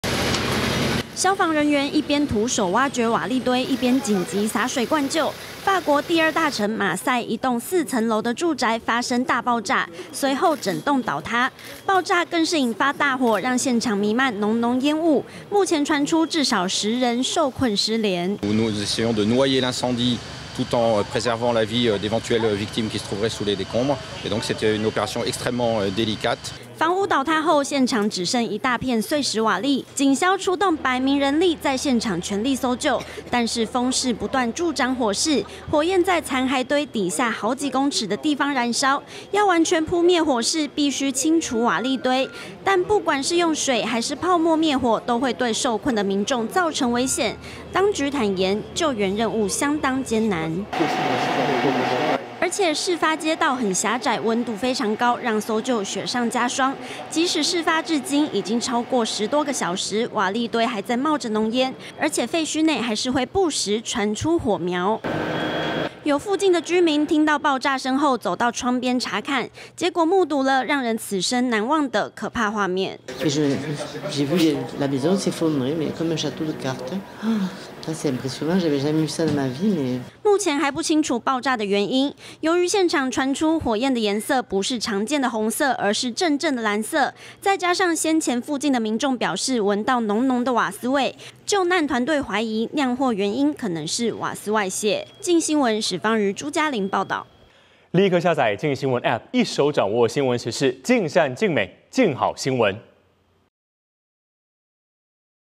法國馬賽住宅爆炸倒塌 傳10人受困生死不明｜鏡新聞
法國第二大城馬賽昨天（4/9）有一棟住宅先是爆炸，而後整棟倒塌，傳出至少有10人失聯，受困在瓦礫堆底下，目前生死不明。當地居民表示，聽到爆炸聲後，空氣中開始瀰漫大量的煙塵和煤氣味，不少人嚇得衝上街逃命。